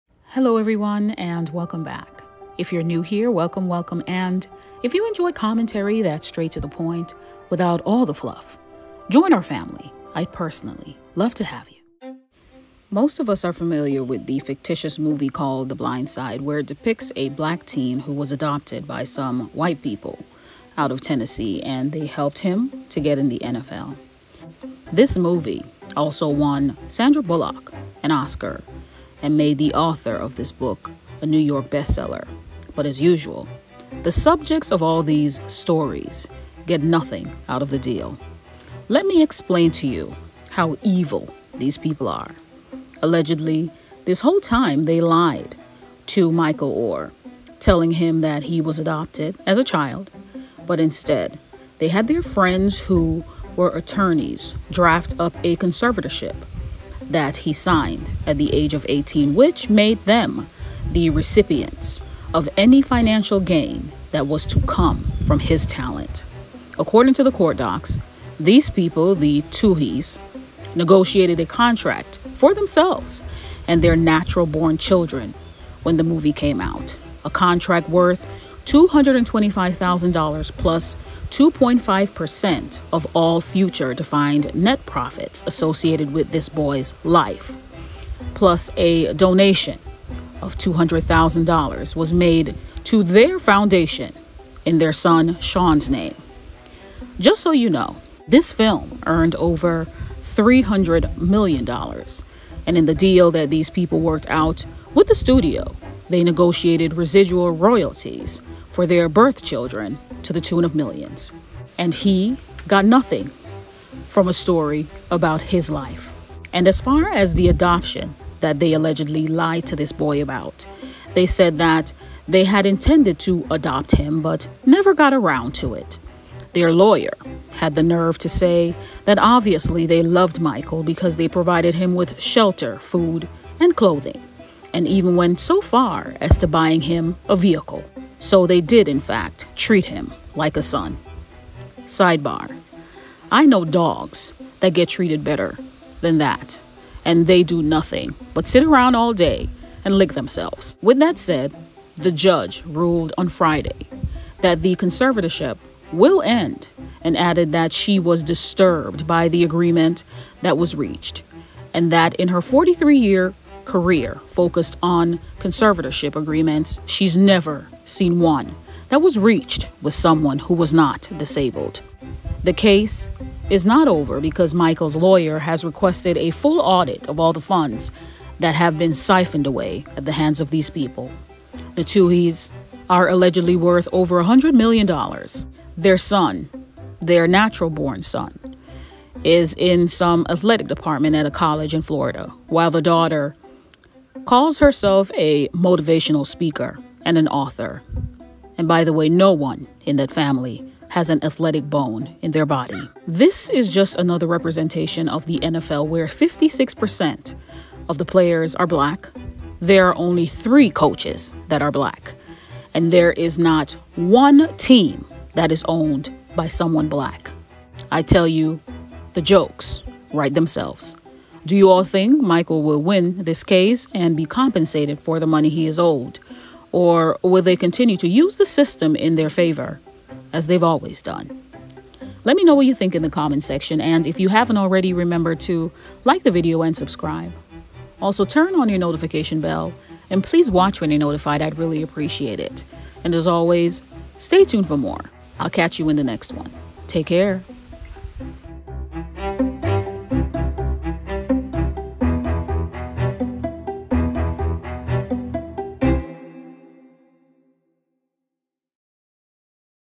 Blindside's Michael Oher BLINDSIDED || They Lied to this Man
Michael Oher 37, recently took the Tuohy's to court pertaining to years of what is alleged to be financial manipulation among other things. The details of this ongoing case are not only shocking but continue to be a standard business practice in the NFL and this country when it comes to BLACK talent and who profits. Also, thank you for your continued support.